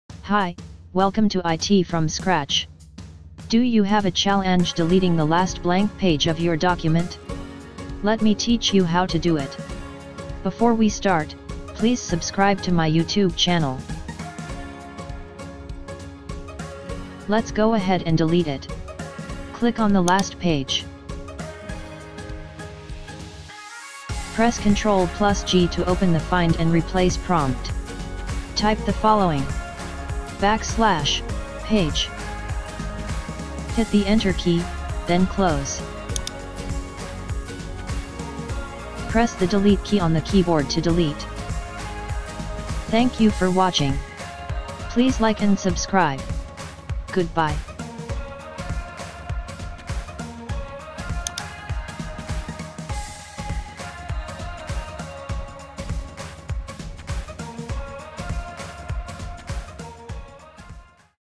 How to Delete a Blank Page You Can't Delete in Word
Discover the method for removing an uneditable blank page in Microsoft Word. Recently, while editing a template document, I encountered a situation where a blank page seemed impossible to delete. In this video, I showcase an uncomplicated solution to resolve this issue.